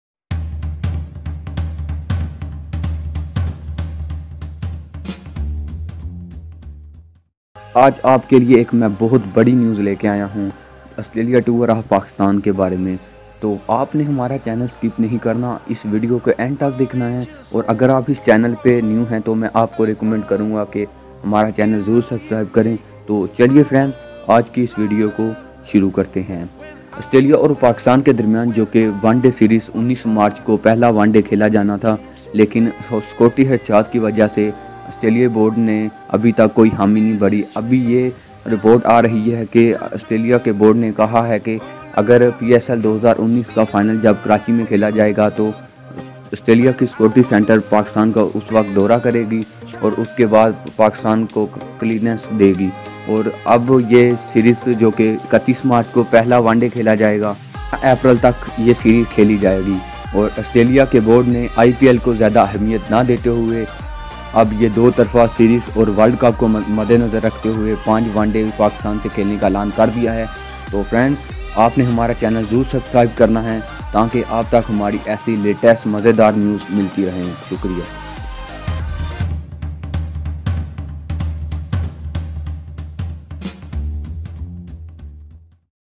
Australia Tours of Pakistan in March 2019| Aus Vs Pak 5 ODI series in Pakistan
COMMENTS
Australia consider touring Pakistan in 2019. ... The West Indies played a three-match T20 series in Karachi in April this year, while Australian Test captain Tim Paine, Ben Cutting and George Bailey were part of a World XI that played a three-match T20 series there in 2017
click